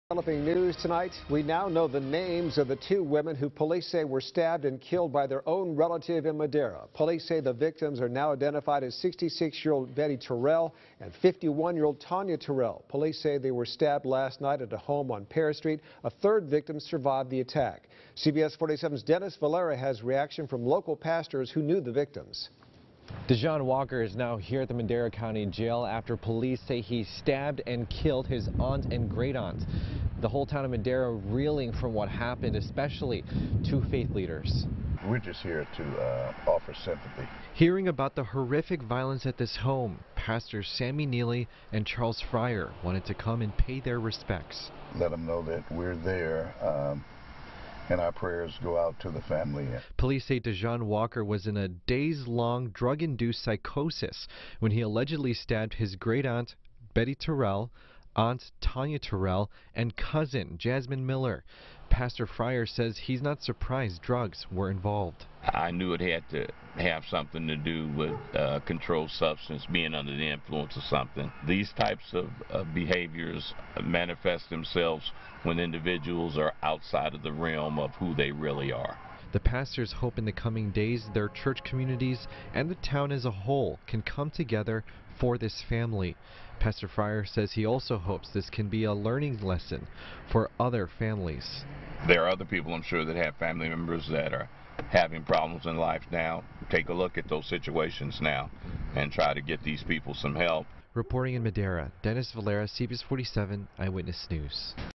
Madera faith leaders want community to unite after fatal stabbings
Two pastors on Thursday drove past the home where Madera Police say three women were stabbed by a relative who was under a drug-induced psychosis just the day before. The pair wanted to pay their respects to the family and hope the community can come together for them.